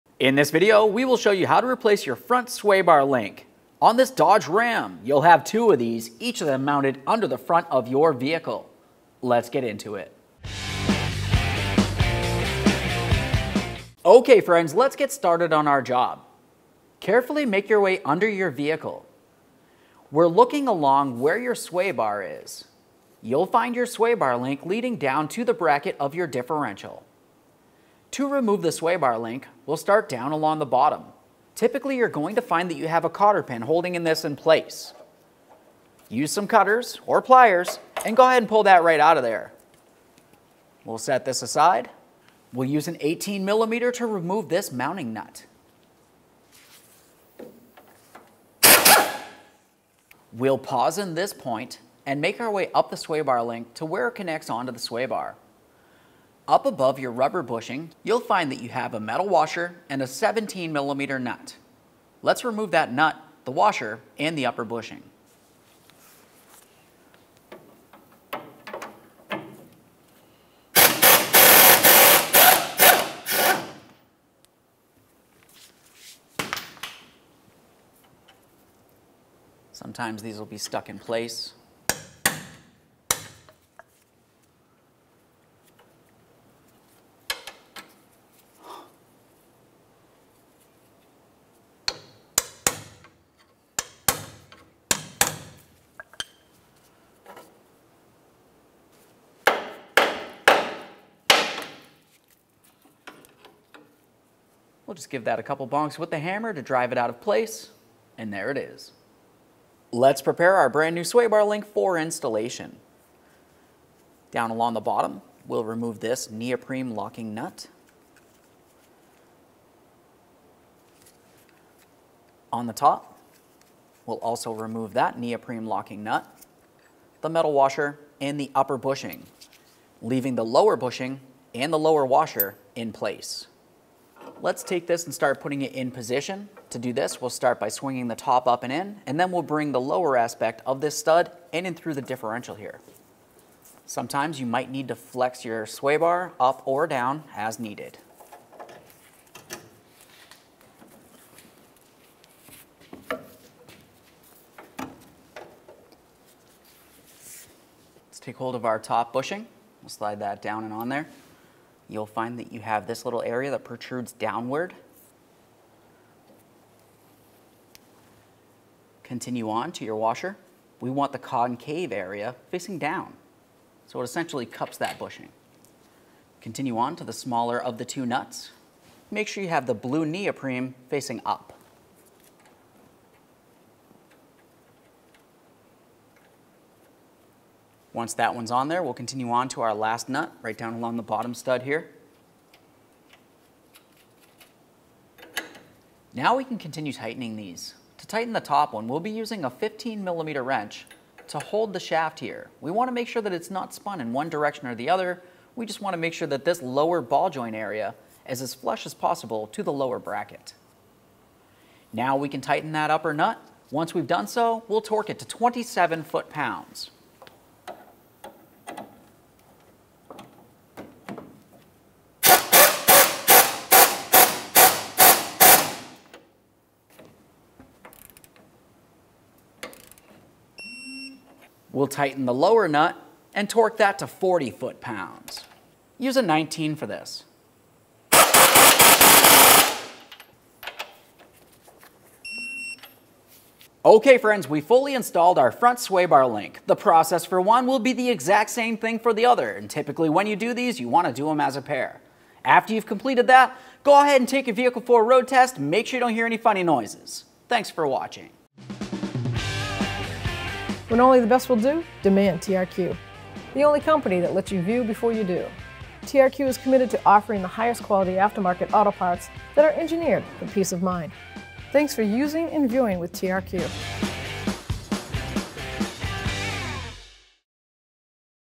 How to Replace Front Sway Bar Links 1994-2002 Dodge Ram 2500
This video shows you how to install new front sway bar links from TRQ on your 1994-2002 Dodge Ram 2500. Sway bar links connect the sway bar (stabilizer bar) to other suspension components. They have a movable joint that can be damaged or wear out over time. When that happens, the joints get loose, and rattle.

This repair was done on a 2002 Dodge Ram 2500 Base 5.9L Extended Cab Pickup 4-Door 4WD Automatic and the process should be similar on the following vehicles:
1994 Dodge Ram 2500
1995 Dodge Ram 2500
1996 Dodge Ram 2500
1997 Dodge Ram 2500
1998 Dodge Ram 2500
1999 Dodge Ram 2500
2000 Dodge Ram 2500
2001 Dodge Ram 2500
2002 Dodge Ram 2500

• 18mm Socket
• 19mm Socket
• Side Cutters
• Safety Glasses
• Hammer